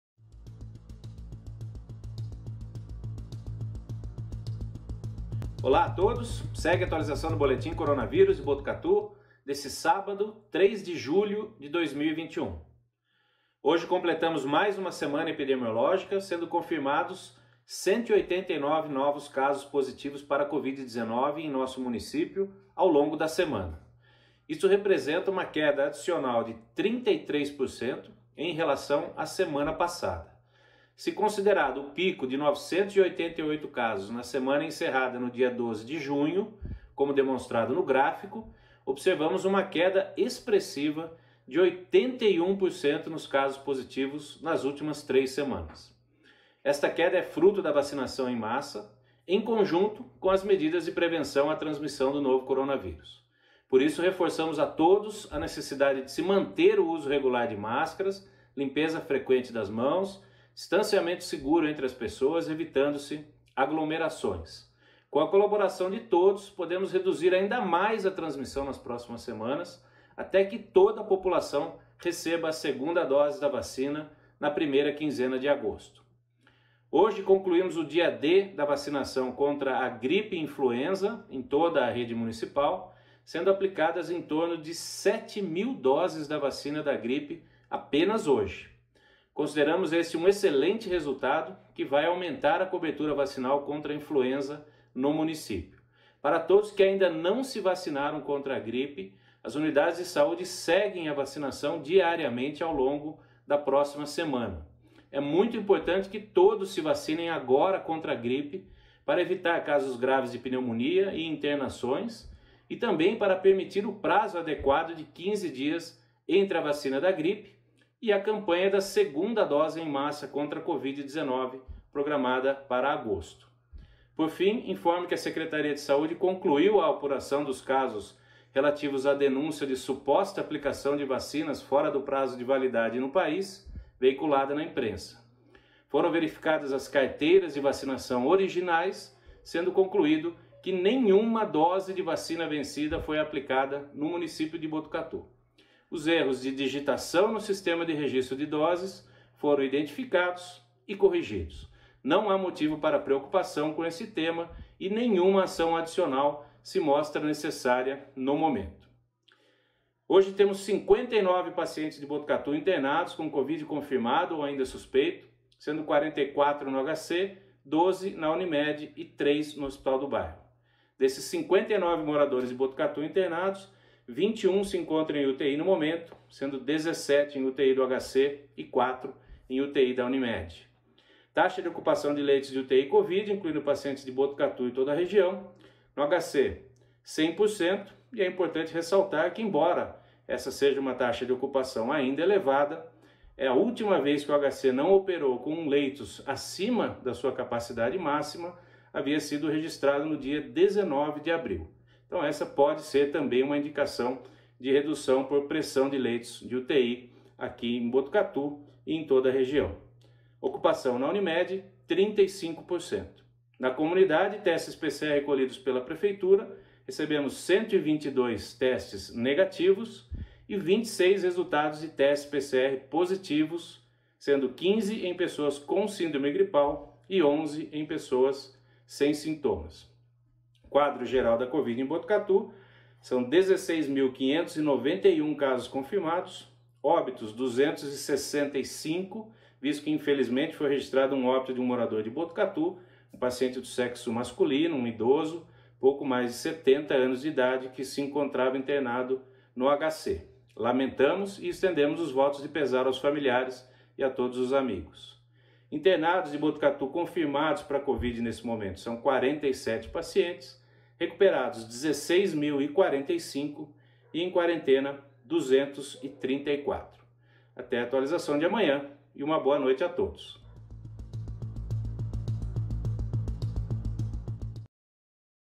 Botucatu registra queda de 81% em casos de Covid 03 07 21
Informe do Coronavírus divulgado pela prefeitura com o resumo semanal.